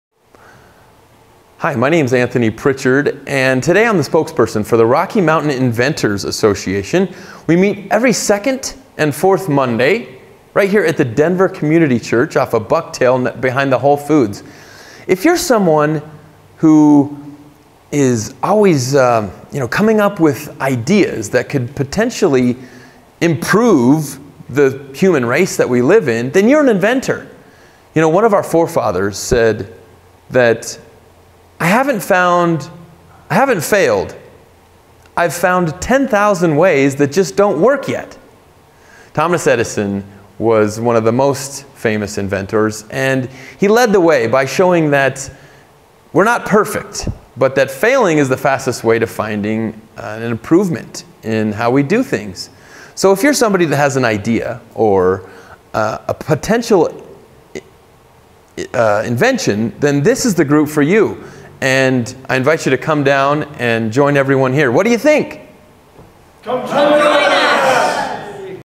April 27, 2015 Anthony Prichard on Social Media for Inventors Club in Denver, CO
If your looking for inventors club in Denver, CO come visit our group.

In this video we talk about our group - When and where it meets.

Rocky Mountain Inventors Group
209 Kalamath St. #9
Denver, CO 80223